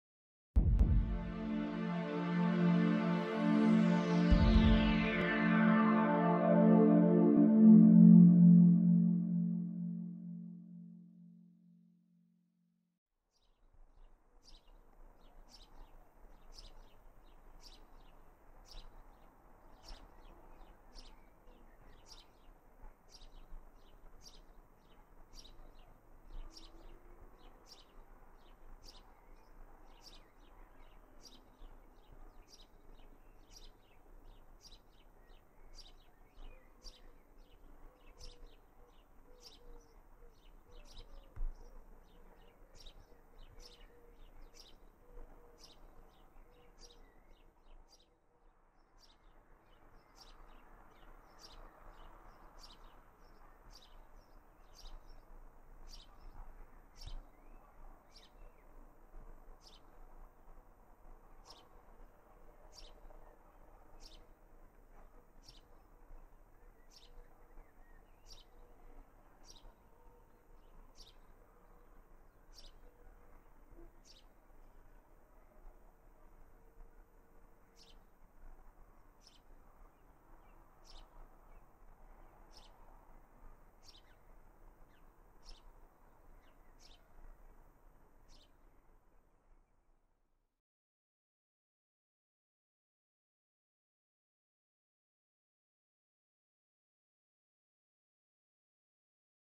Garden Wildlife - House Sparrow 19.06.2018
A young House Sparrow calling on, filmed in my parents garden in Raunds. Filmed on the 19th June 2018.